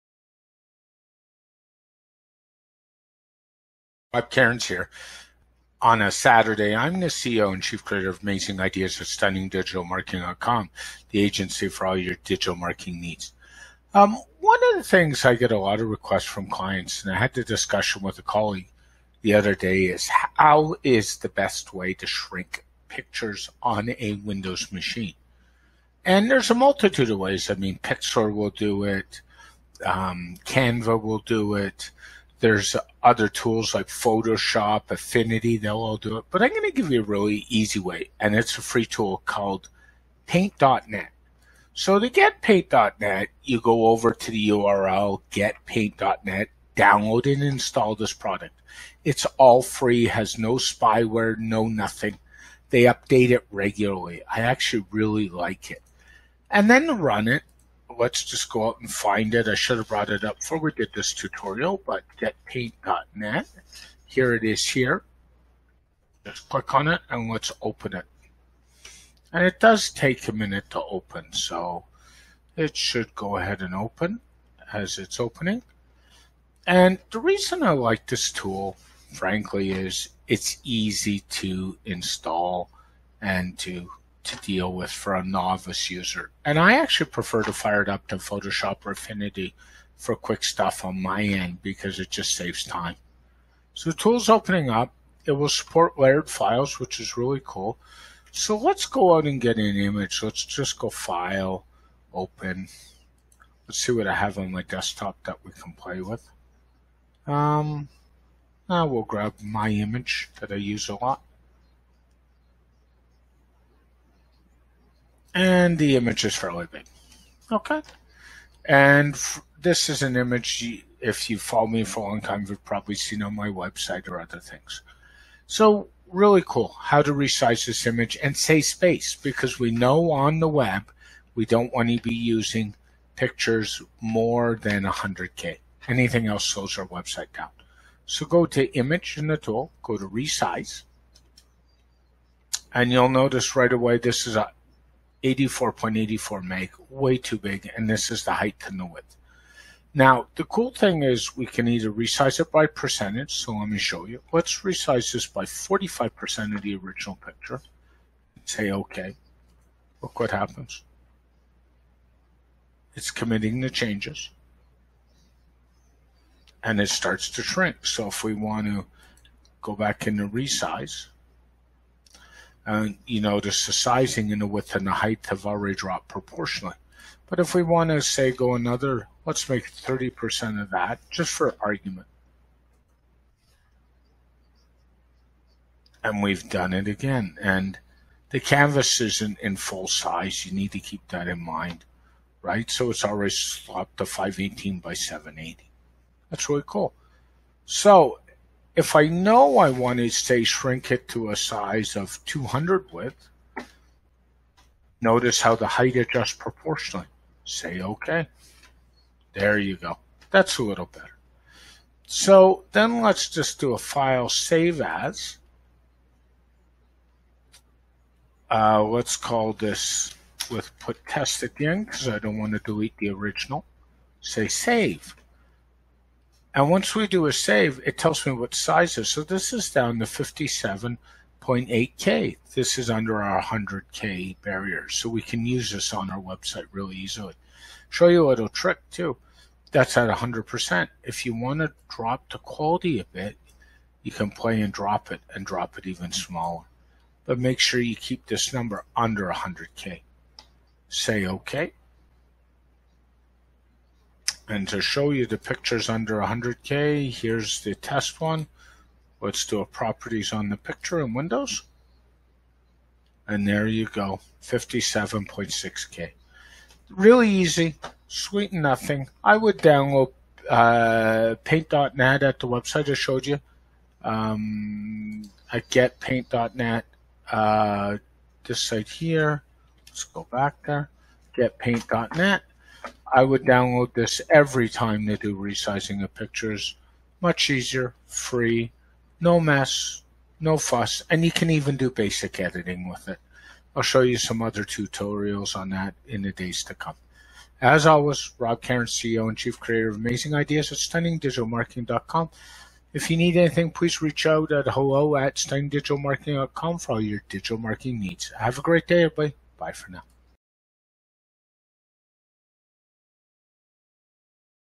How to Resize Photos Using the FREE Paint Net on Windows
An easy way to resize photos using the free Paint.net